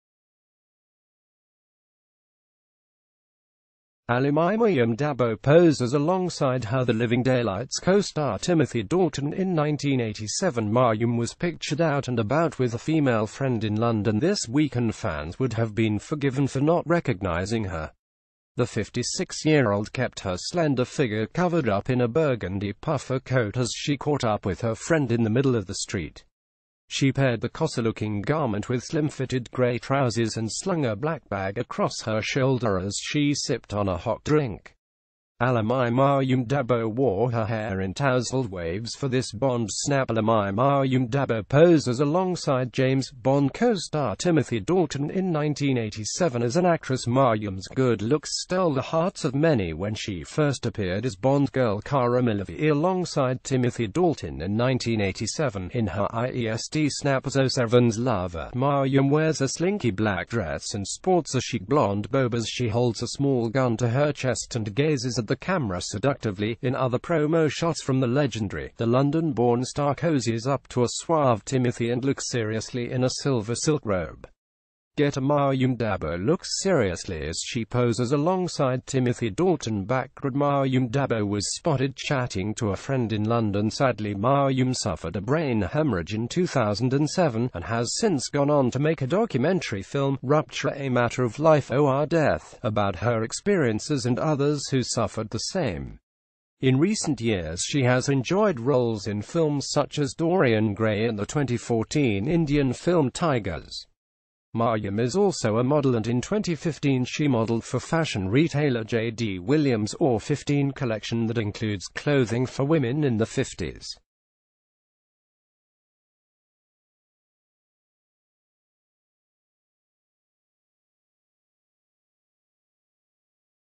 You won’t believe what bond girl maryam d’abo looks like now…| Breaking News TV
ALAMYMaryam dAbo poses alongside her The Living Daylights costar Timothy Dalton in 1987Maryam was pictured out and about with a female friend in Londo...